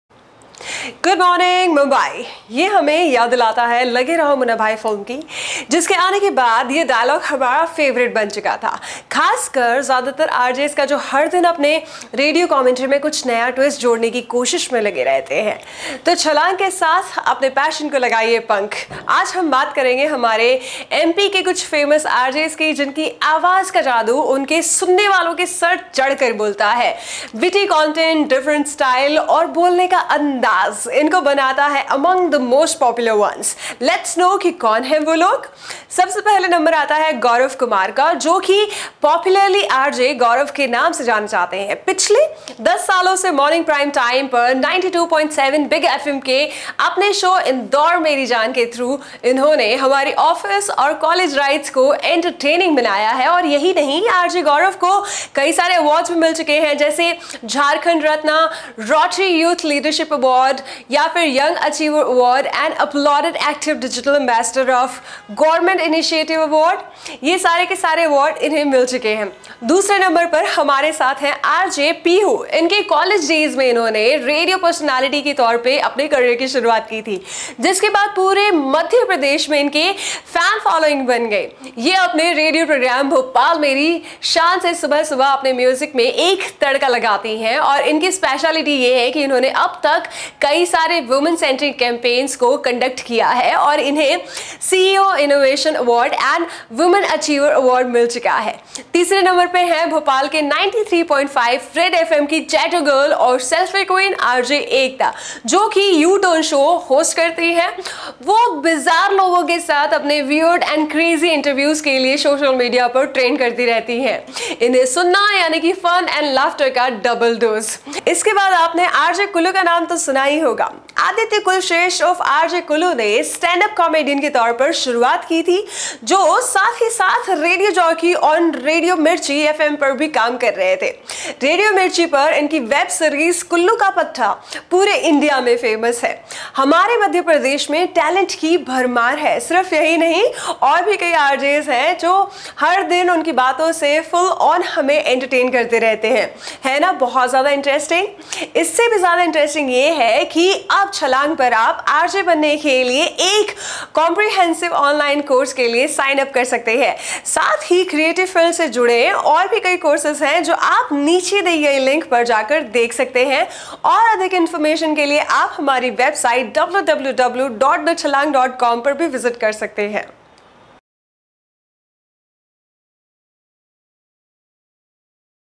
Famous RJs from Madhya Pradesh Who Have Won Our Hearts | Best Radio Jockeys in MP
In this video, Learn about Famous RJs from Madhya Pradesh Who Have Won Our Hearts | Best Radio Jockeys in MP

The earliest means of communication is radio. Radio has its own followers, and radio DJs have their own fans, even in the age of television and YouTube. A radio jockey's duty is to use his or her voice to amuse, educate, and enlighten a large audience. Casting a spell over people with just your voice needs a lot of inventiveness and energy. Take a peek at these ten well-known Madhya Pradesh radio personalities.

Here is a list of ten well-known Madhya Pradesh radio jockeys that have captured our hearts and continue to amuse us on a daily basis.

Share and learn what you're passionate about.